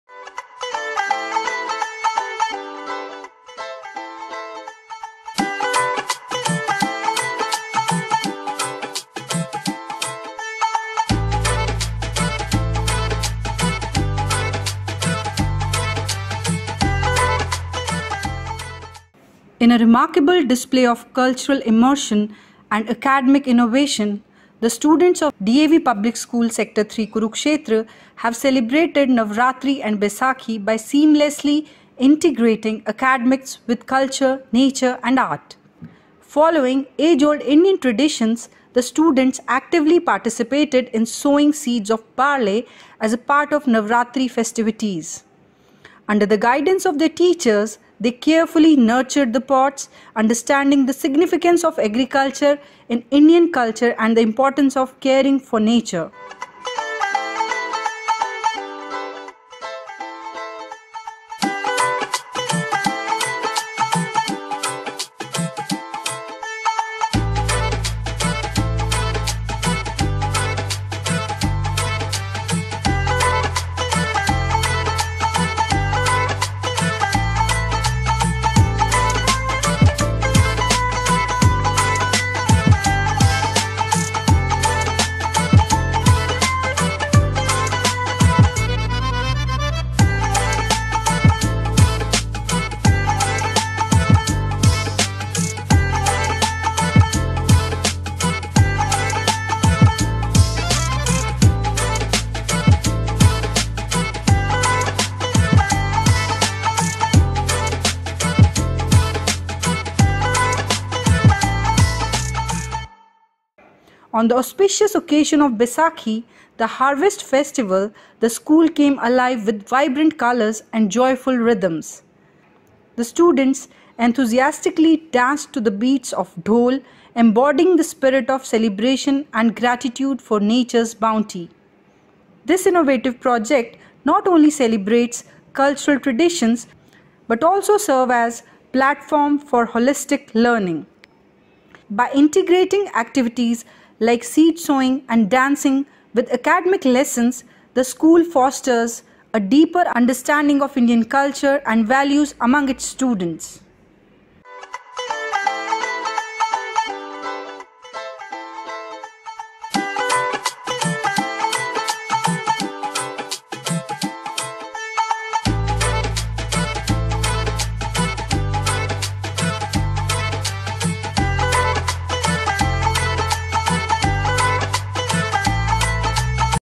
Navratri and Baisakhi Celebrations | April 2024
DAV Public School, Sector-3, Kurukshetra, Pioneers Integration of Academics, Culture, and Art with Navratri and Baisakhi Celebrations

Kurukshetra, April 15, 2024 – In a remarkable display of cultural immersion and academic innovation, the small students of DAV Public School, Sector-3, Kurukshetra, have celebrated Navratri and Baisakhi by seamlessly integrating academics with culture, nature, and art.

Following age-old Indian traditions, the students actively participated in sowing seeds of barley as part of the Navratri festivities. Under the guidance of their teachers, they carefully nurtured the pots, understanding the significance of agriculture in Indian culture and the importance of caring for nature.

On the auspicious occasion of Baisakhi, the harvest festival, the school came alive with vibrant colors and joyful rhythms. The students enthusiastically danced to the beats of the dhol, embodying the spirit of celebration and gratitude for nature's bounty.

This innovative project not only celebrated cultural traditions but also served as a platform for holistic learning. By integrating activities like seed sowing and dancing with academic lessons, the school fostered a deeper understanding of Indian culture and values among its students.

Mrs. Geetika Jasuja, Principal of DAV Public School, Sector-3, Kurukshetra, expressed her delight at the success of the project, stating, "Our aim is to provide students with a well-rounded education that goes beyond textbooks. By integrating academics with cultural activities, we not only nurture their academic abilities but also instill a sense of pride in their heritage."

Mrs. Vandana Sardana, (Coordinator) said "The project's significance is further underscored by its alignment with the National Education Policy (NEP) 2020, which emphasizes the integration of arts and culture into the education system. By prioritizing art integration, DAV Public School, Sector-3, Kurukshetra, sets a shining example for educational institutions across the country."

As the students continue to reap the benefits of this holistic approach to learning, the school remains committed to nurturing young minds and fostering a deep appreciation for India's rich cultural heritage.

In a world where academic excellence often takes precedence over cultural understanding, initiatives like these serve as beacons of hope, reminding us of the importance of a well-rounded education that celebrates both the mind and the spirit.